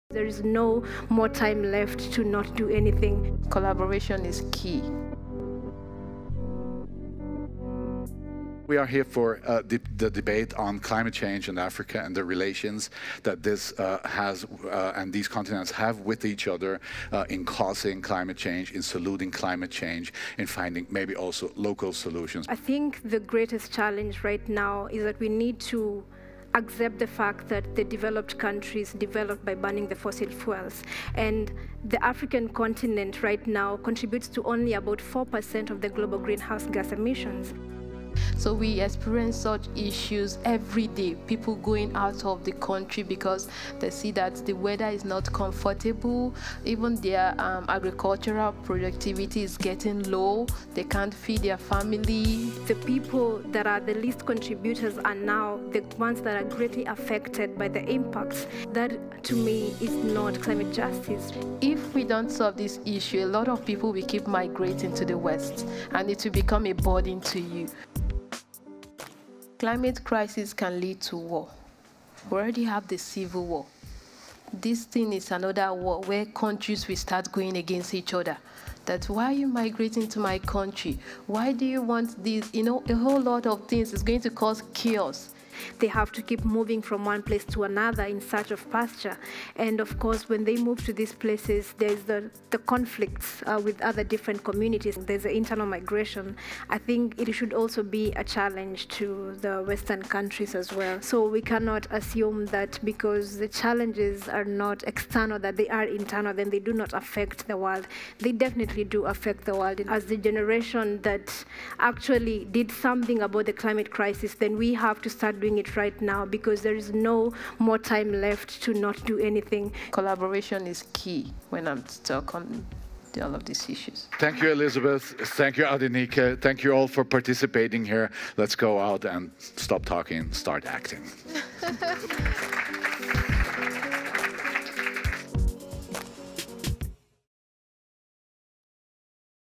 Elevate 2020 Klimakrise: Fokus Afrika
Mein spannendes Gespräch mit zwei starken Frauen aus Afrika: Adenike Oladosu ist nigerianische Klima-Aktivistin, Öko-Feministin und die Initiatorin der Bewegung "Fridays For Future" in Nigeria. Elizabeth Wathuti ist kenianische Umwelt- und Klima-Aktivistin und Gründerin der Green Generation Initiative, die junge Menschen schon in jungen Jahren zur Liebe zur Natur und zum Umweltbewusstsein erzieht.